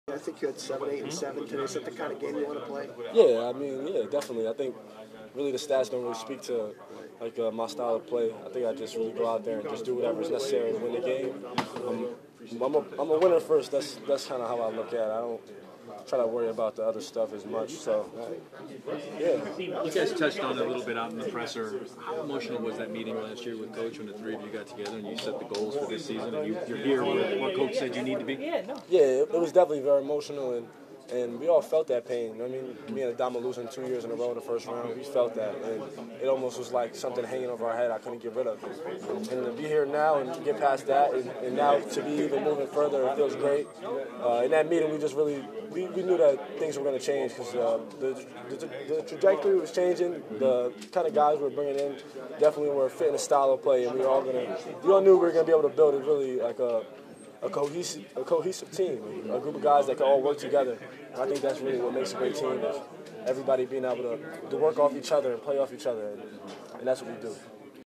UConn's Andre Jackson reacts to Sweet 16 win over Arkansas | Full Interview
Jackson had a solid all-around game in their win with seven points, eight rebounds and seven assists.